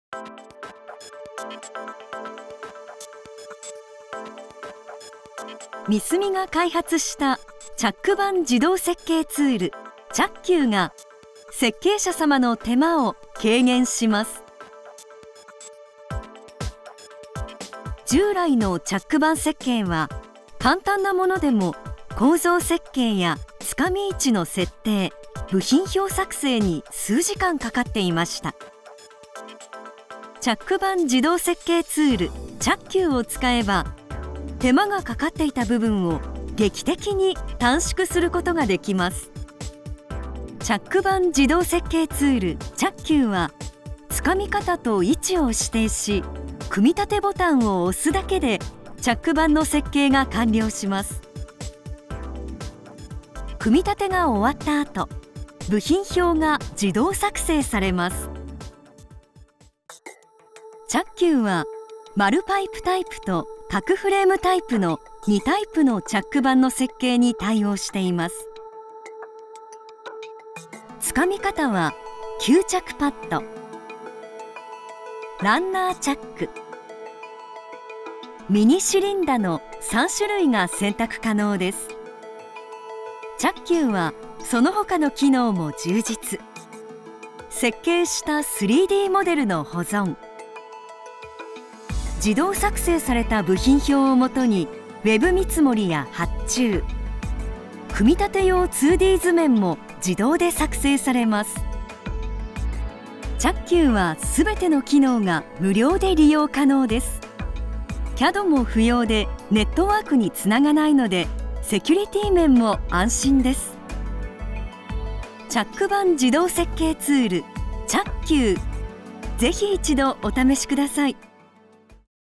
ミスミ チャック板自動設計ツール「CHUC-Q(チャッキュー)」
チャック板自動設計ツール「CHUC-Q(チャッキュー)」のご紹介。

【概要】
成形品データを取り込んでいただくと、チャック板を瞬時に自動設計いたします！

【設計ステップ】
①成形品データを取りこむ
②つかみ部品、位置を指定
③1クリックで自動的に部品を配置、組立
④部品表を自動生成
⑤部品表を出力しWOSで見積もり、発注